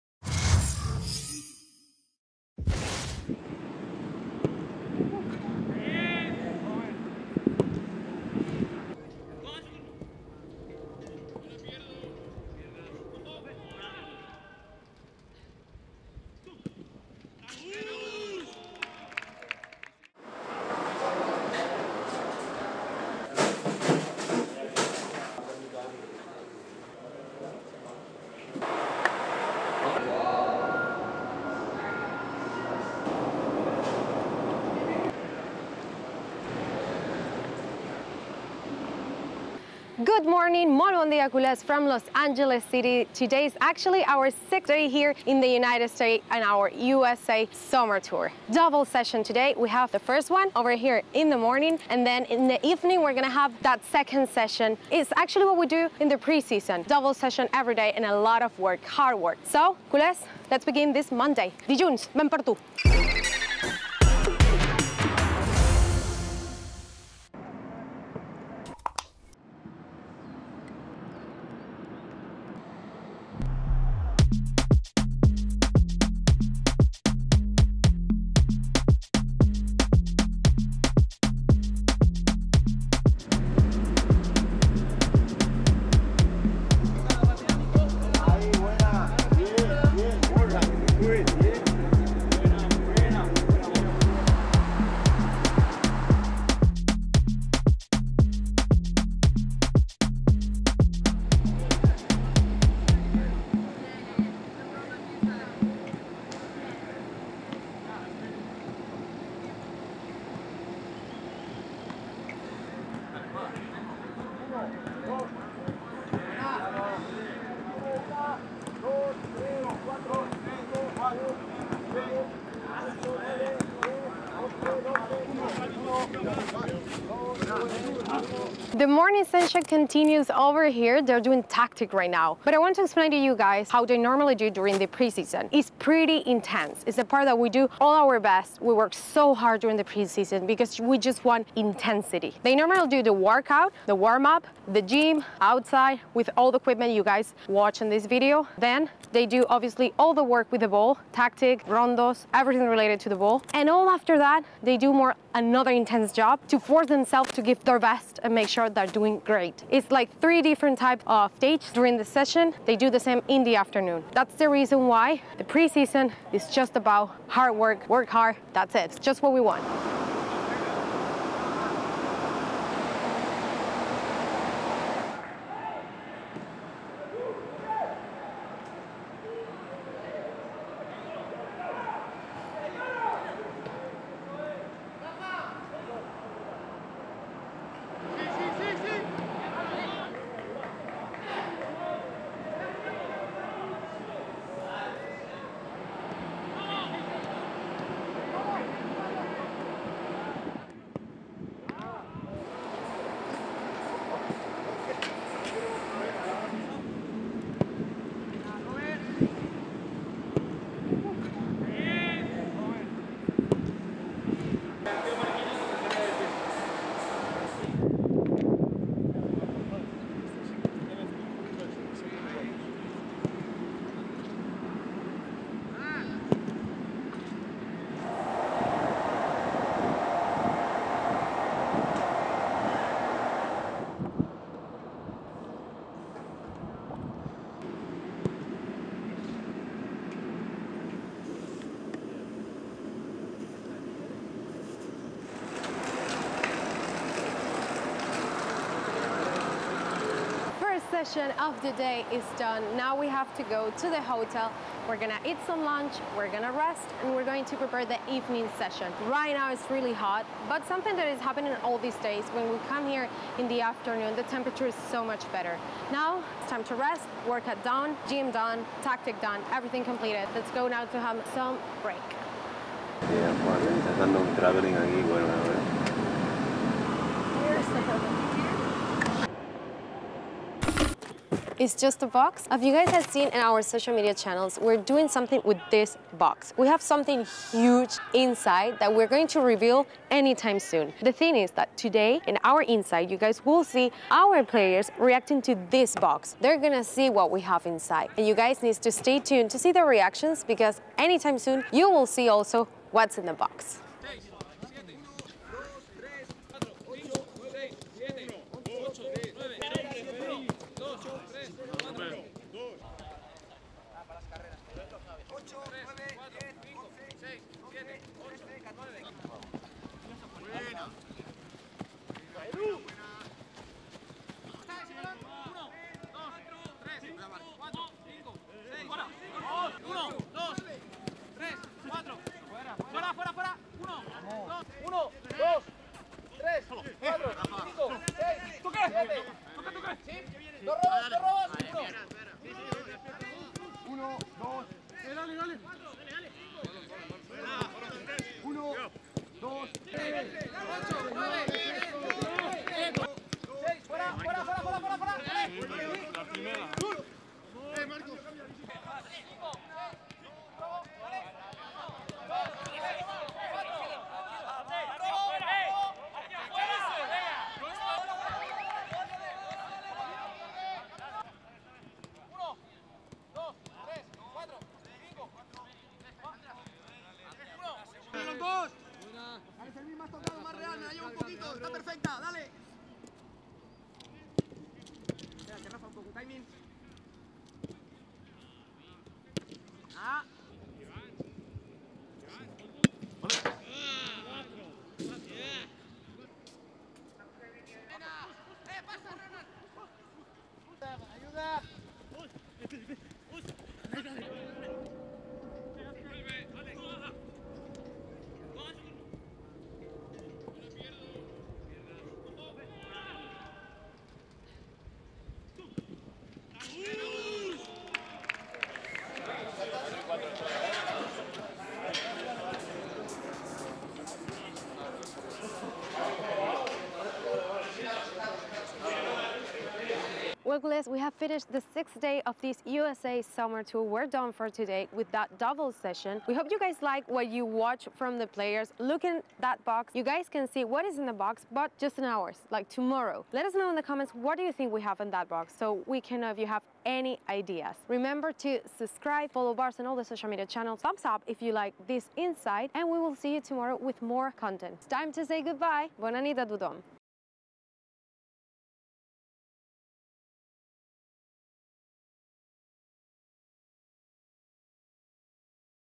TRAINING X 2 + OUR SPECIAL BOX 💙❤️ | INSIDE TOUR (day 6)
Monday ushered in a long-awaited return to normalcy for FC Barcelona. With the viral gastroenteritis that had forced the cancellation of the preseason opener final no longer lingering, the Catalans took part in a pair of rigorous training sessions. Once again on the hallowed grounds of the Los Angeles Memorial Coliseum, the epicenter of Barça's stateside summer tour, Xavi Hernández's players continued fine tuning their their game on American soil.